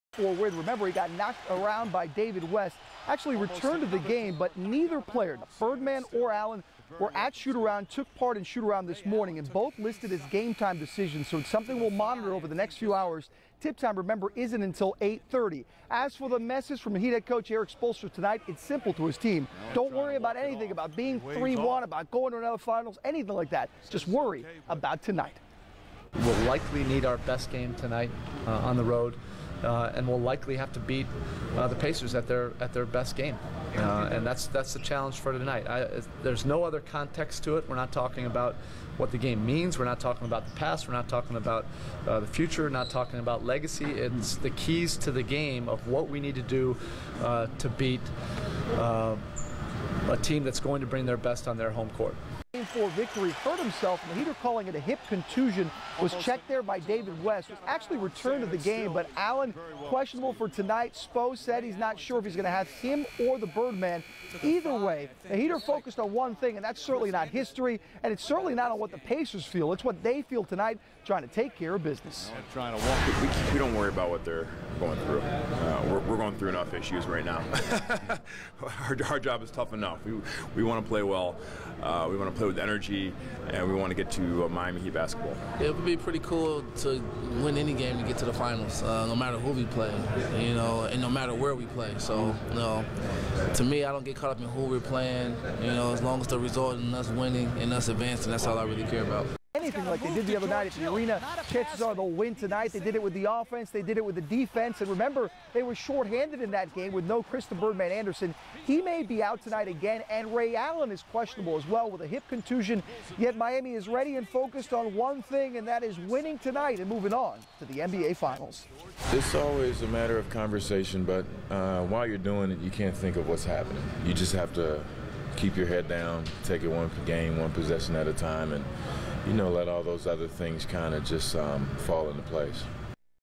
May 28, 2014 - WPLG 10 - Miami Heat Prior to 2014 NBA East Conf Finals Game 05 (@ Pacers)
May 28, 2014 -  WPLG 10 - Miami Heat Prior to 2014 NBA Eastern Conference Finals Game 05 (@ Indiana Pacers)